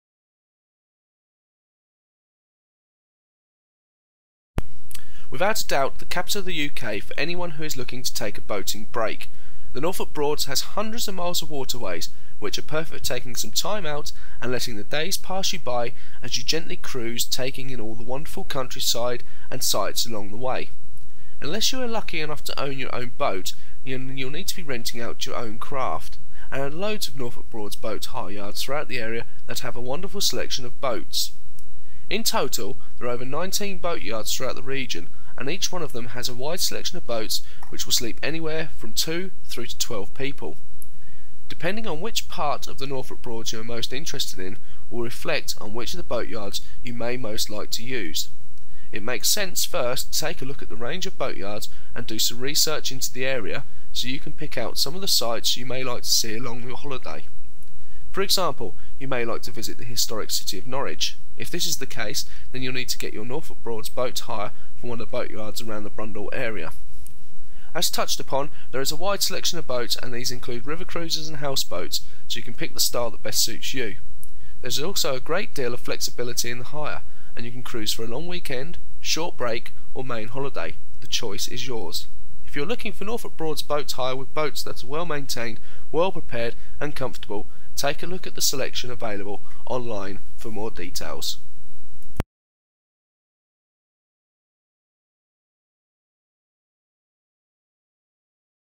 Boat Hire Norfolk Broads - 19 Boatyard Locations
- Boat Hire Norfolk Broads is one of the most popular choices for boating holidays in the UK.  This video show you the sights and the boats you could be enjoying for your next holiday on the waterways in Norfolk.